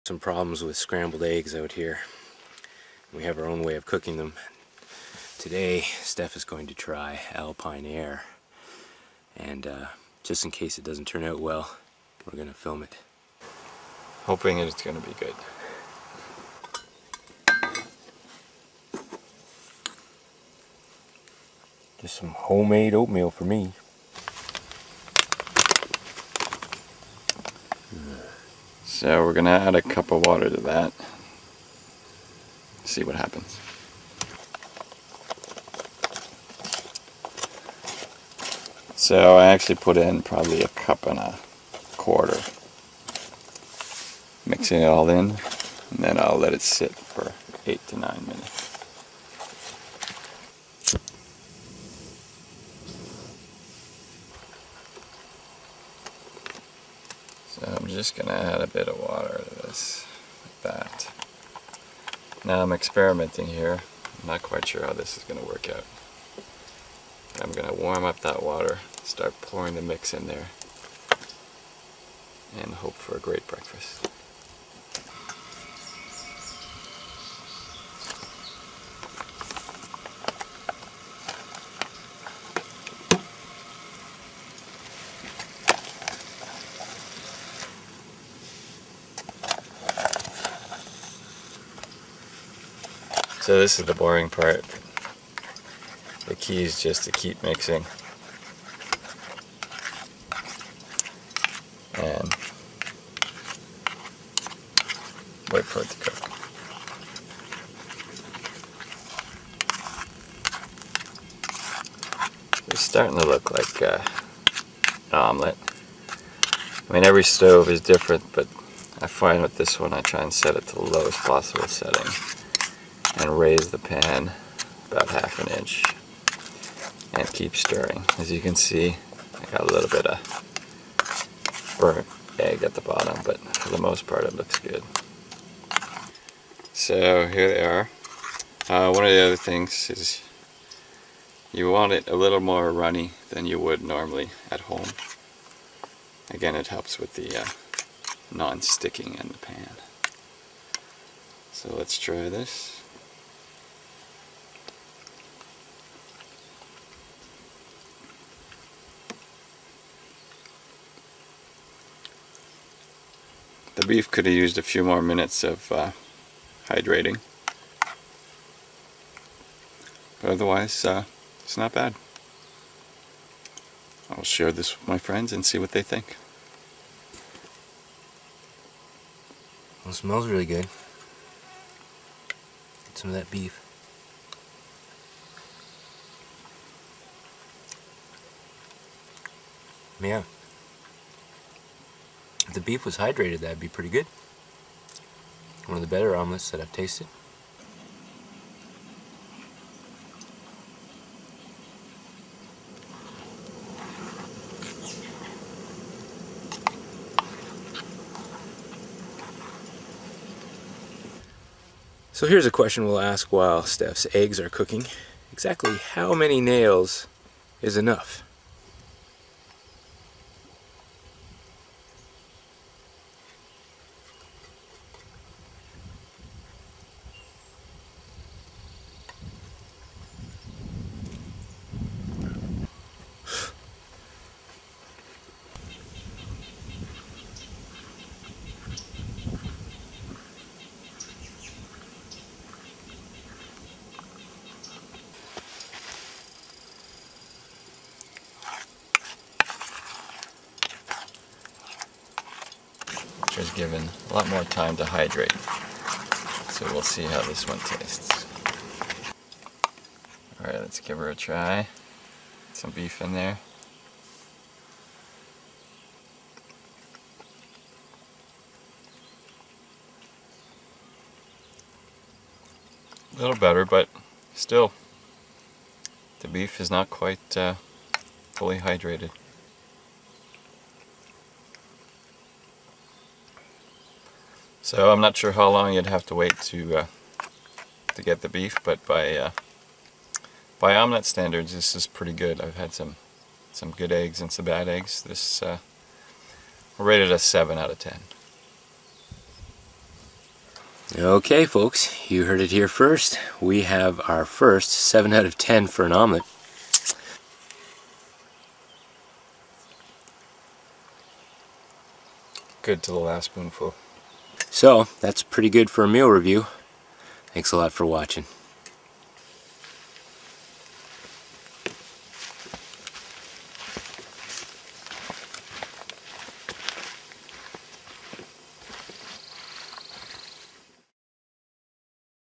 Ranch Omelet with Beef - AlpineAire Foods - BackCountry Meal Review
Ranch Omelet with Beef by AlpineAire Foods rated 7 out of 10. In this video we also ask viewers, "How many nails is enough?"  Please comment if you have tried this meal or make a suggestion of one you like better.  You may see your suggestion in our next video.  The setting for this review is Round Schooner Lake inside beautiful North Frontenac Parklands, Canada. Please rate this video.  Thanks!

Freeze Dried Camp Food Meal Review.  This video series gets better with your participation.